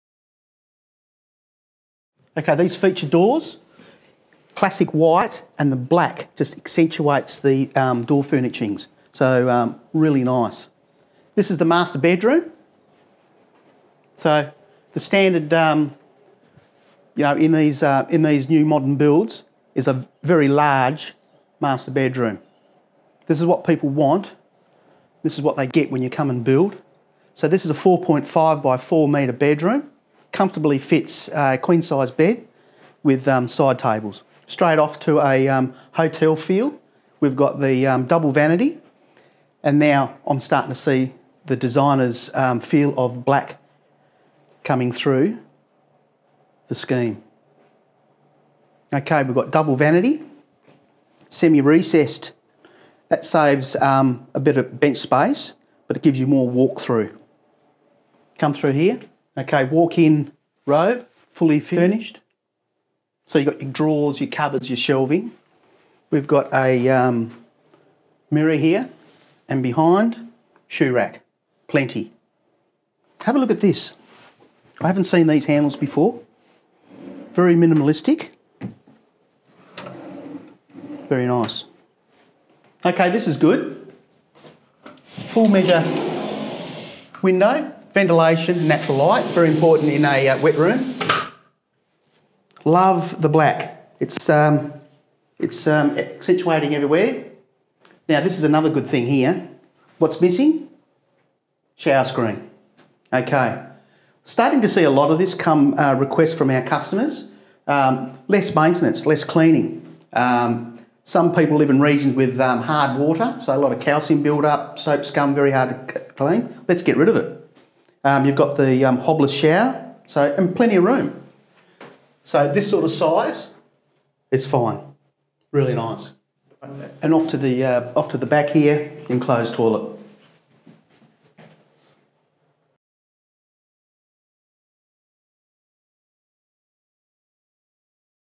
Master Suite [B1 Homes - The Lena Display]
Watch Matt take a walk through B1 Homes The Lena display home.
Here we take a closer look at the master bedroom and ensuite.